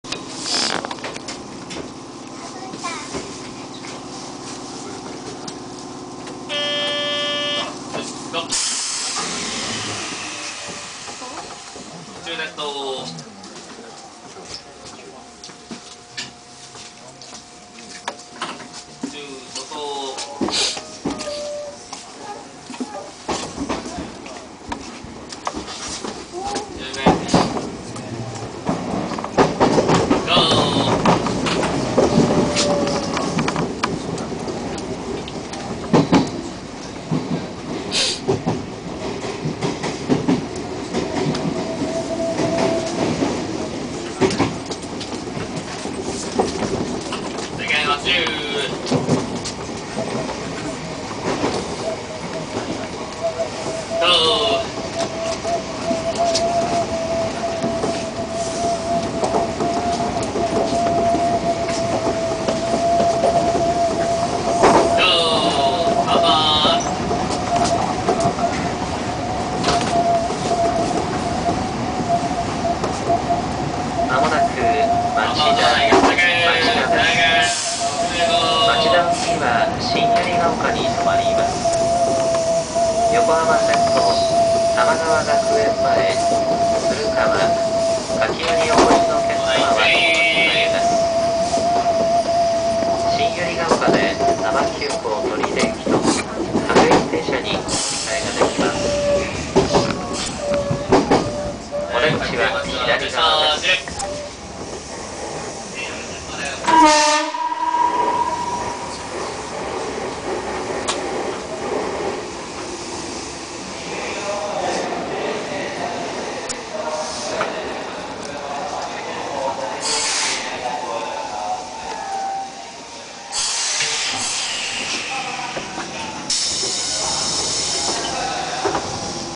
小田急電鉄快速急行前面展望5/10 相模大野～町田
小田急電鉄江ノ島・小田原線の前面展望です。快速急行の8000形未更新車で撮影しました。